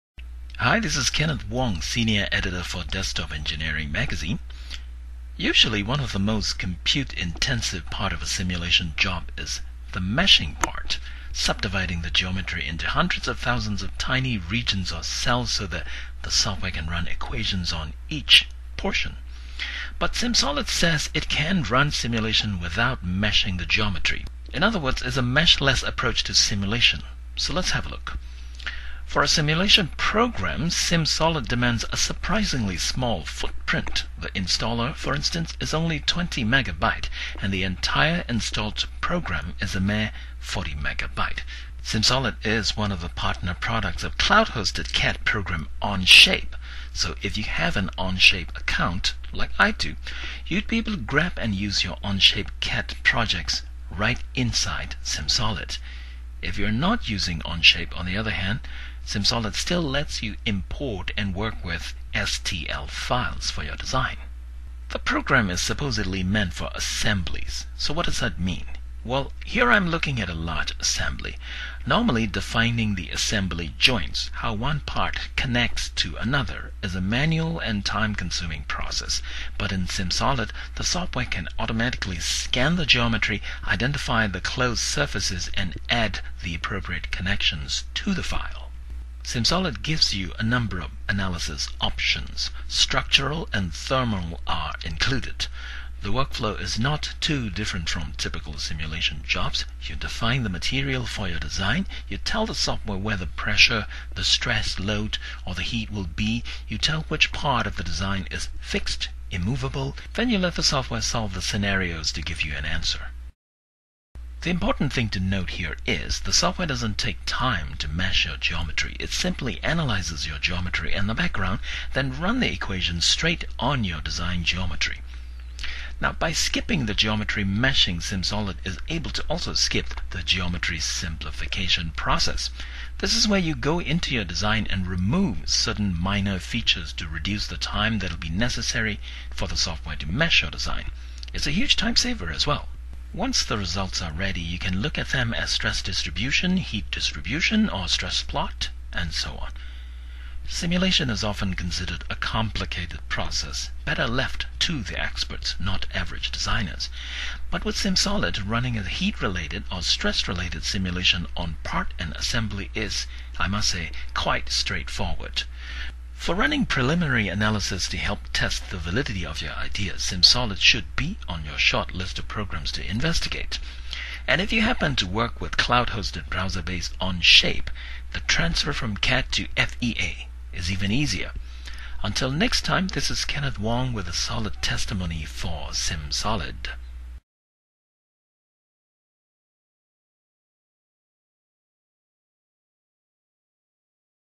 Meshless FEA with SIMSOLID
DE editor Kenneth Wong takes a look at SIMSOLID's meshless simulation